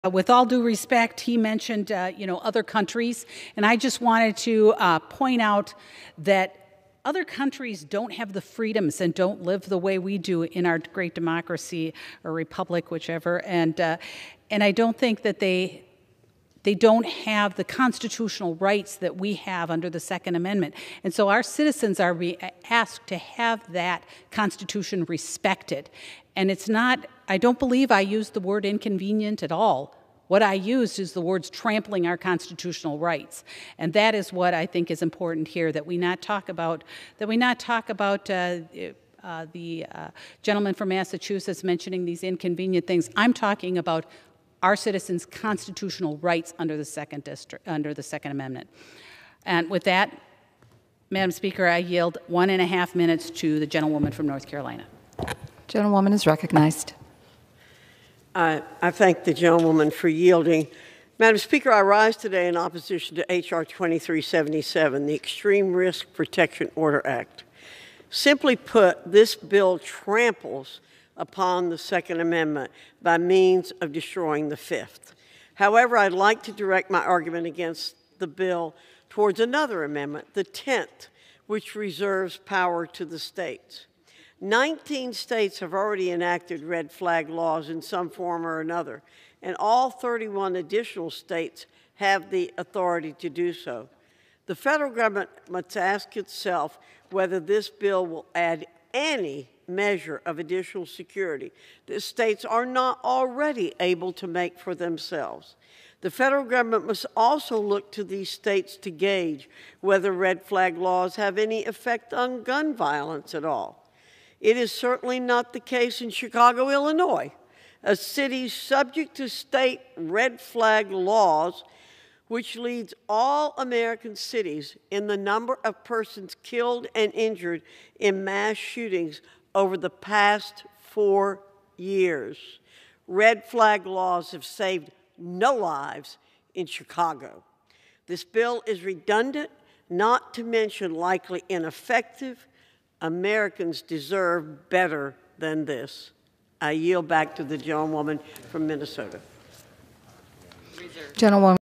Virginia Foxx: 'This Bill Tramples On The Second Amendment'
In remarks on the House floor last week, Rep. Virginia Foxx (R-NC) spoke about the Second Amendment.